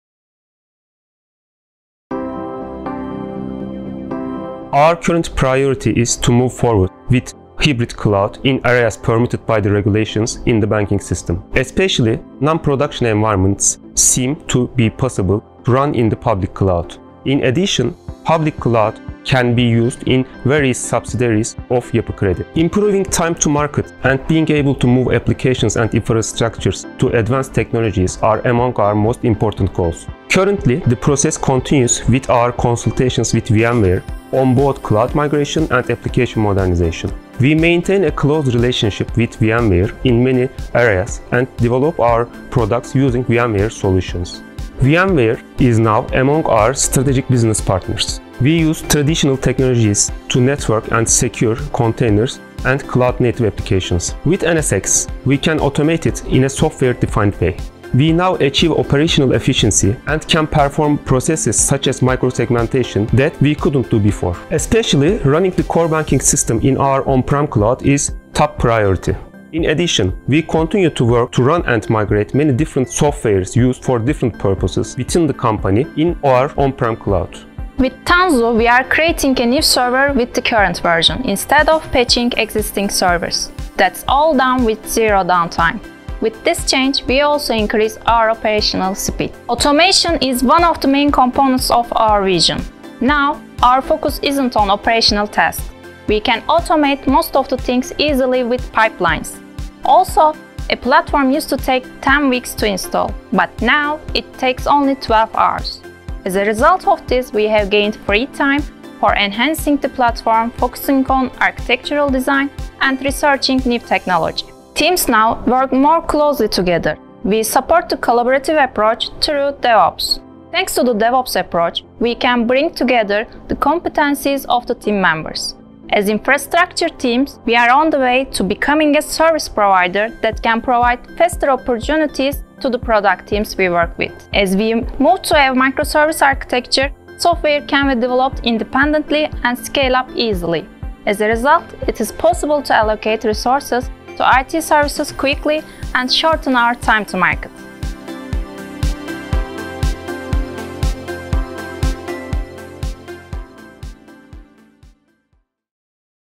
Yapi Kredi Bank is migrating apps for a better customer experience
The third largest private bank in Turkey, Yapı Kredi is increasing its in-house application development, agility, and end user satisfaction by placing VMware at the center of its modern IT infrastructure.